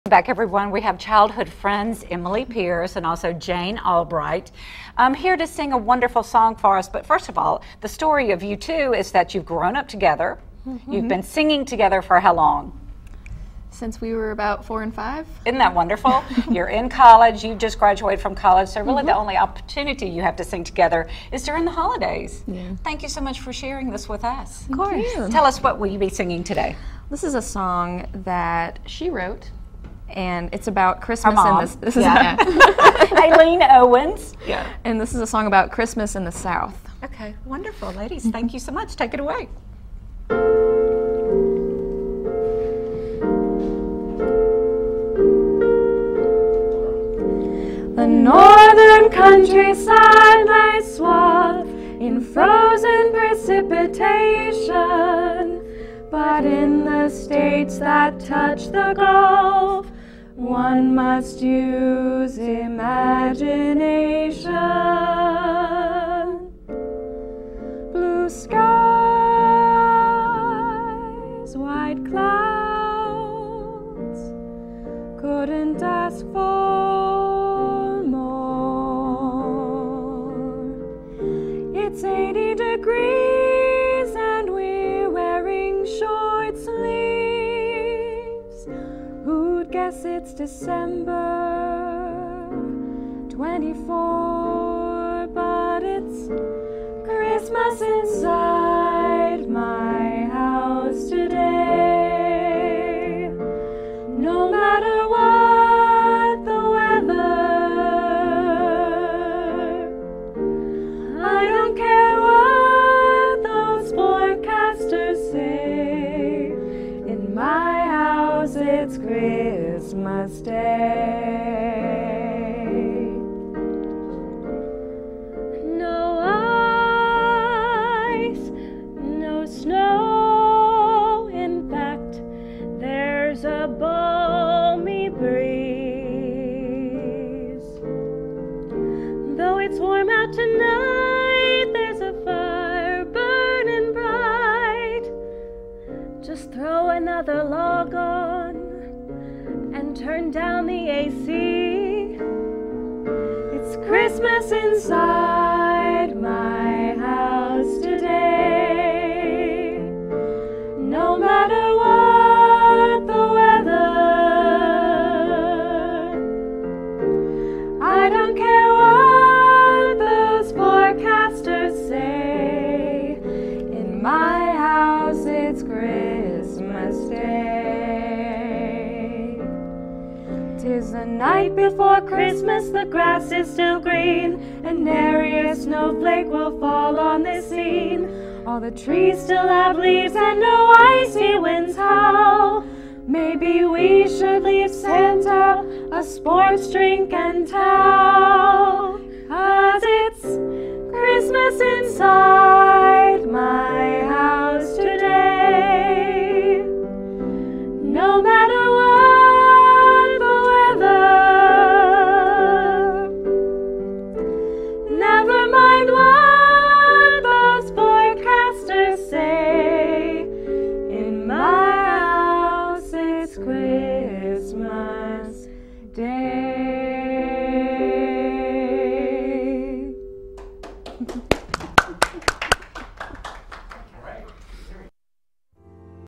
Emily Pears and Jane Albright sing
Good Day Cenla special Christmas show. Emily and Jane with  their own holiday arrangement.